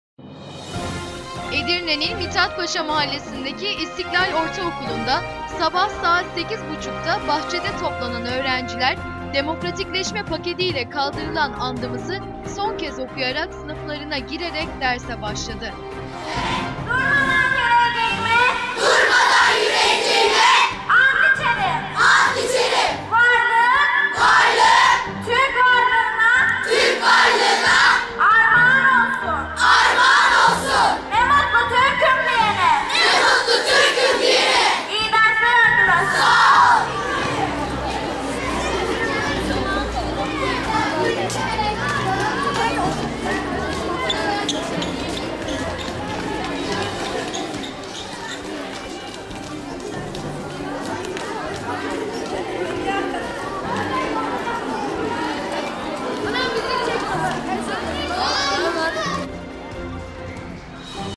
Edirne'de Andımız Son Kez Okundu
EDİRNE de  SON KEZ OKUNDU
 Edirnenin mithatpaşa Mahallesindeki İstiklal Orta Okulunda sabah 08.30?da bahçede toplanan ö»renciler demokratikleşme paketiyle kaldırılan Andımız' ı son kez okuyarak sınıflarına girerek derse başladı.